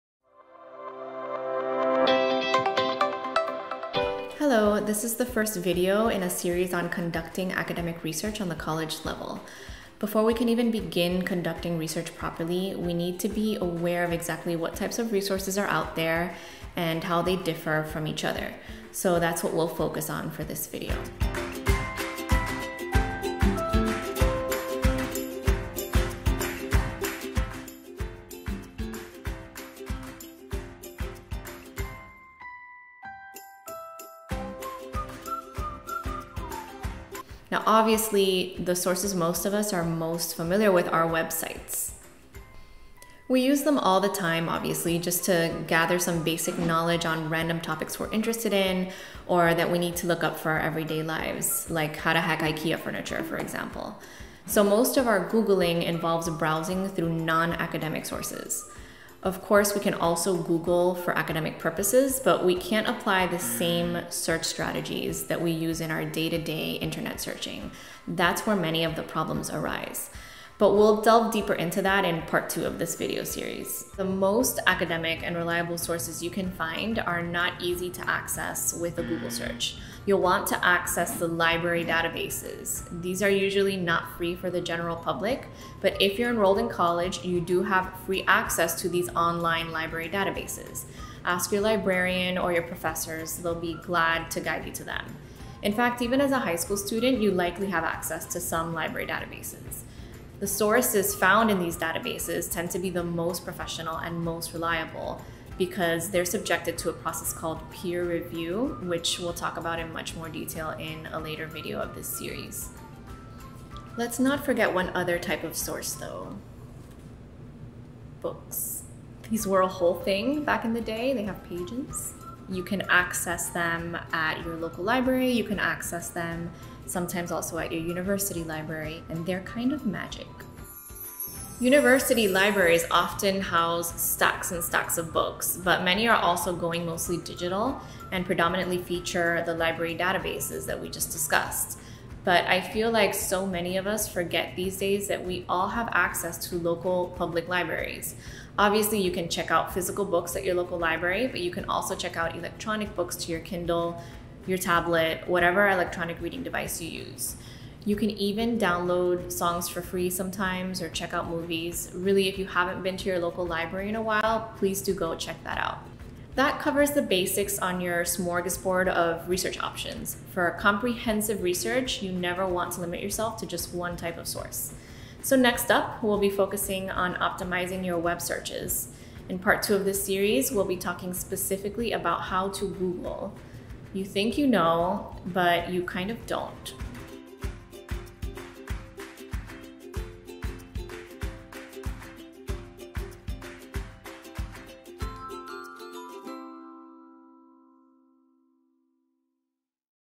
West Coast University - Conducting Academic Research: Types of Academic Sources
In this first video of the series “Conducting Academic Research”, Professor Melissa Cueto introduces the many types of research out there for you to discover! This video covers:

- Types of sources
- Ways to make use of local libraries

Want to see the rest of the video series? Check these out!

Curious about West Coast University and our other learning content? Check out @West Coast University